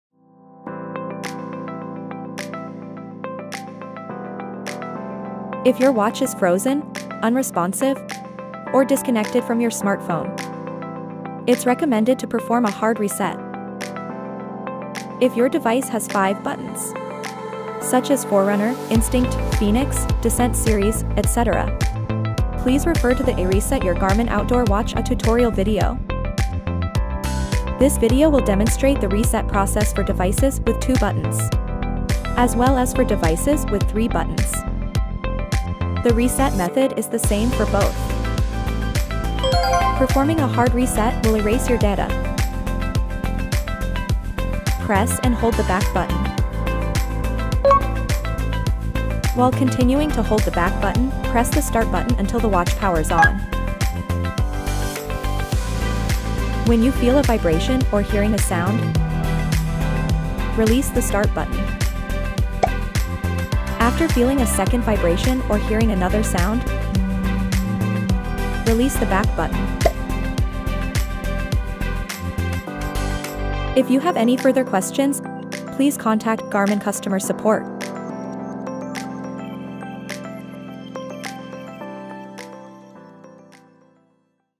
[튜토리얼] Garmin 시계 리셋하기: 2개 및 3개 버튼 모델
시계가 멈췄거나 반응이 없거나 스마트폰과의 연결이 끊어진 경우
하드 리셋을 수행하는 것이 좋습니다.
장치에 Forerunner, Instinct, fēnix, Descent 시리즈 등 5개의 버튼이 있는 경우
'Garmin 아웃도어 워치 리셋하기' 튜토리얼 비디오를 참조하십시오.